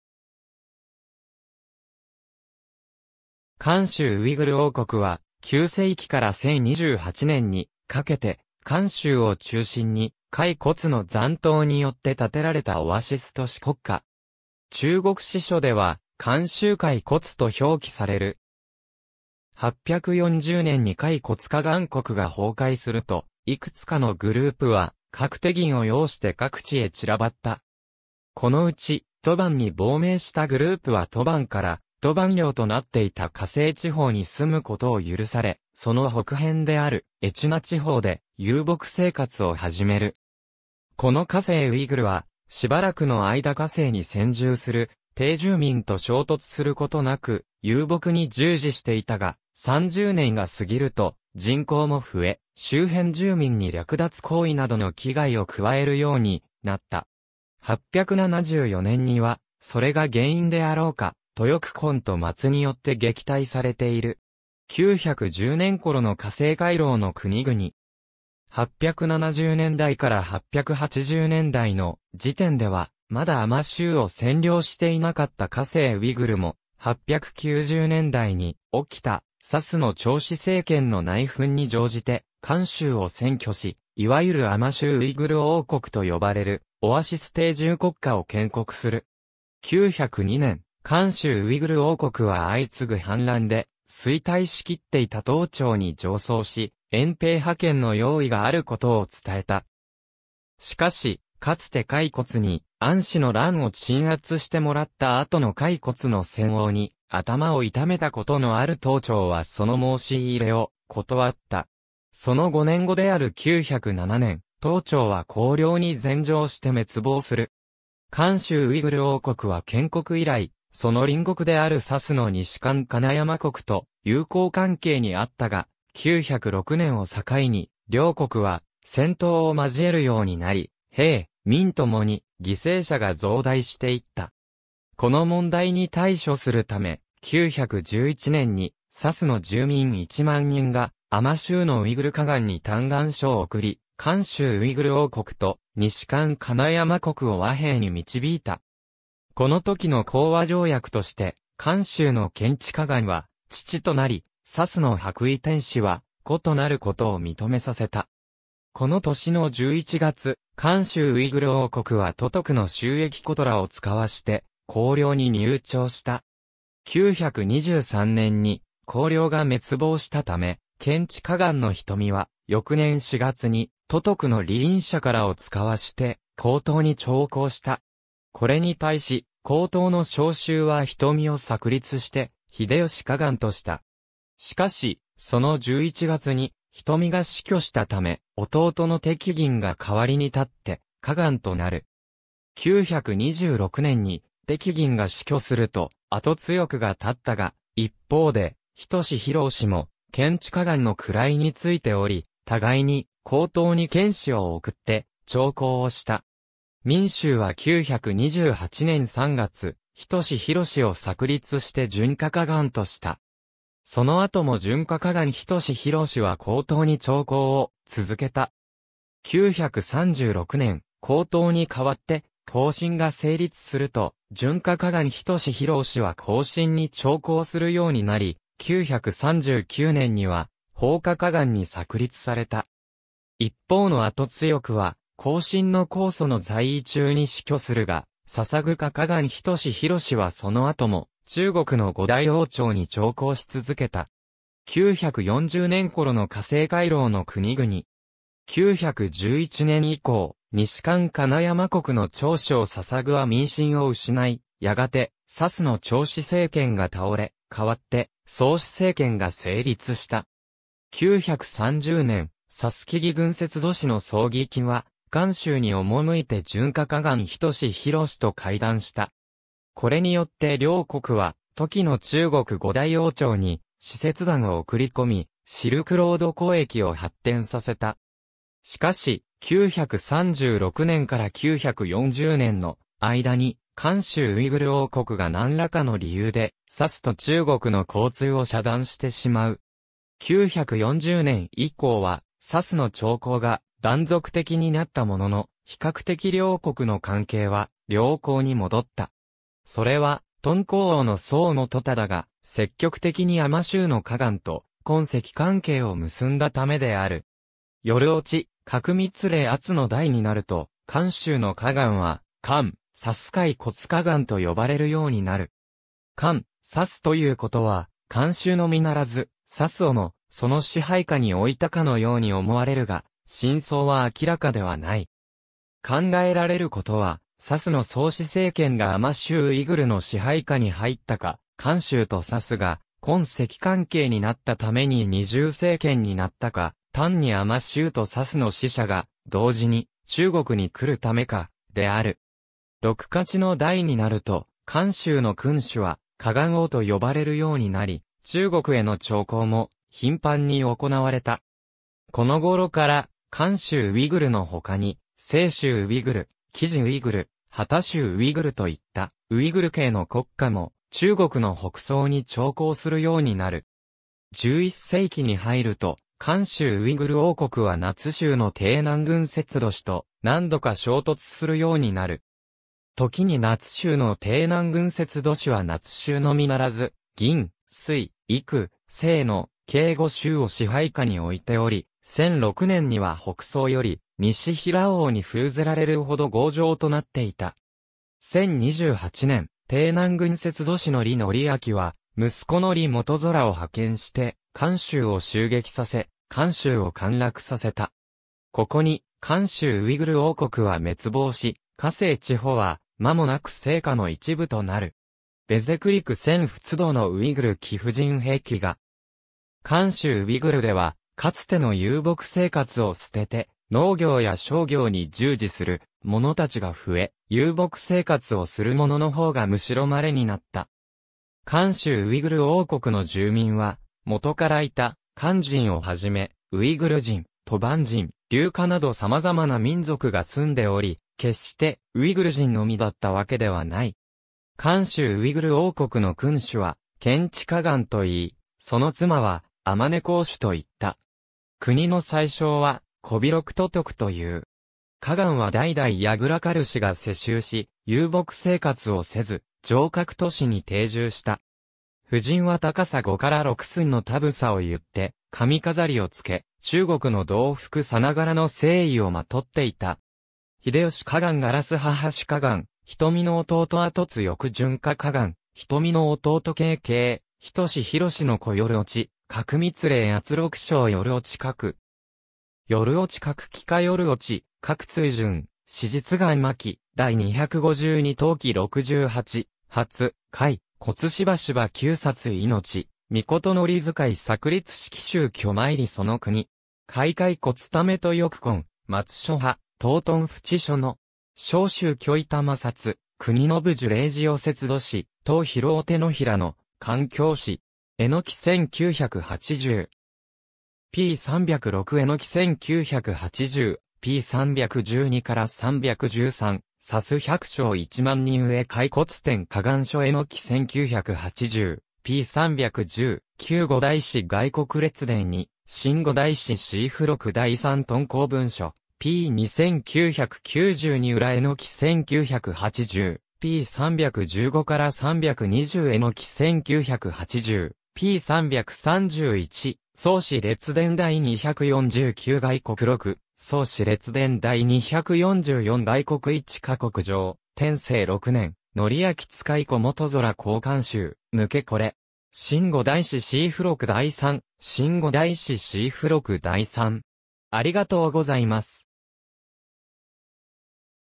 甘州ウイグル王国
甘州ウイグル王国（かんしゅうウイグルおうこく）は、9世紀から1028年にかけて甘州（現在の中華人民共和国甘粛省張掖市）を中心に、回鶻（ウイグル）の残党によって建てられたオアシス都市国家。
中国史書では甘州回鶻と表記される。
840年（開成5年）に回鶻可汗国が崩壊すると、いくつかのグループは各テギン（王子）を擁して各地へ散らばった。
このうち、吐蕃に亡命したグループは吐蕃から吐蕃領となっていた河西地方に住むことを許され、その北辺であるエチナ地方で遊牧生活を始める。
この河西ウイグルはしばらくの間河西に先住する定住民と衝突することなく遊牧に従事していたが、30年が過ぎると人口も増え、周辺住民に掠奪行為などの危害を加えるようになった。
874年にはそれが原因であろうか、吐谷渾と嗢末によって撃退されている。
910年頃の河西回廊の国々。
870年代～880年代の時点ではまだ甘州を占領していなかった河西ウイグルも、890年代に起きた沙州（現在の敦煌市）の張氏政権の内紛に乗じて甘州を占拠し、いわゆる甘州ウイグル王国（甘州回鶻）と呼ばれるオアシス定住国家を建国する。
902年（天復2年）、甘州ウイグル王国は相次ぐ叛乱で衰退しきっていた唐朝に上奏し、援兵派遣の用意があることを伝えた。
しかし、かつて回鶻に安史の乱を鎮圧してもらった後の回鶻の専横に頭を痛めたことのある唐朝はその申し入れを断った。
その5年後である907年（唐:天祐4年、後梁:開平元年）、唐朝は後梁に禅譲して滅亡する。
甘州ウイグル王国は建国以来、その隣国である沙州の西漢金山国と友好関係にあったが、906年（天祐3年）を境に両国は戦闘を交えるようになり、兵・民ともに犠牲者が増大していった。
この問題に対処するため、911年（開平5年）に沙州の住民1万人が甘州のウイグル可汗に嘆願書を送り、甘州ウイグル王国と西漢金山国を和平に導いた。
このときの講和条約として、甘州の権知可汗（仁美）は父となり、沙州の白衣天子（張承奉）は子となることを認めさせた。
この年の11月、甘州ウイグル王国は都督の周易言らを遣わして後梁に入朝した。
923年（後梁:龍徳3年、後唐:同光元年） に後梁が滅亡したため、権知可汗の仁美は翌年（924年）4月に都督の李引釈迦らを遣わして後唐に朝貢した。
これに対し、後唐の荘宗は仁美を冊立して英義可汗とした。
しかし、その11月に仁美が死去したため、弟の狄銀が代わりに立って可汗となる。
926年（同光4年）に狄銀が死去すると、阿咄欲が立ったが、一方で仁裕も権知可汗の位に就いており、互いに後唐に遣使を送って朝貢をした。
明宗は928年（天成3年）3月、仁裕を冊立して順化可汗とした。
その後も順化可汗仁裕は後唐に朝貢を続けた。
936年（天福元年）、後唐に代わって後晋が成立すると、順化可汗仁裕は後晋に朝貢するようになり、939年（天福4年）には奉化可汗に冊立された。
一方の阿咄欲は後晋の高祖の在位中（936年 - 942年）に死去するが、奉化可汗仁裕はその後も中国の五代王朝に朝貢し続けた。
940年頃の河西回廊の国々。
911年（天祐3年）以降、西漢金山国の張承奉は民心を失い、やがて沙州の張氏政権が倒れ、代わって曹氏政権が成立した。
930年（長興元年）、沙州帰義軍節度使（敦煌王）の曹議金は甘州に赴いて順化可汗仁裕と会談した。
これによって両国は時の中国五代王朝に使節団（キャラバン）を送り込み、シルクロード交易を発展させた。
しかし、936年（天福元年）から940年（天福5年）の間に甘州ウイグル王国が何らかの理由で沙州と中国の交通を遮断してしまう。
940年以降は沙州の朝貢が断続的になったものの、比較的両国の関係は良好に戻った。
それは敦煌王の曹元忠が積極的に甘州の可汗と婚戚関係を結んだためである。
夜落隔密礼遏の代になると、甘州の可汗は甘・沙州回鶻可汗と呼ばれるようになる。
甘・沙州ということは甘州のみならず、沙州をもその支配下に置いたかのように思われるが、真相は明らかではない。
考えられることは、沙州の曹氏政権が甘州ウイグルの支配下に入ったか、甘州と沙州が婚戚関係になったために二重政権になったか、単に甘州と沙州の使者が同時に中国に来るためか、である。
禄勝の代になると、甘州の君主は可汗王と呼ばれるようになり、中国への朝貢も頻繁に行われた。
この頃から甘州ウイグルの他に西州ウ...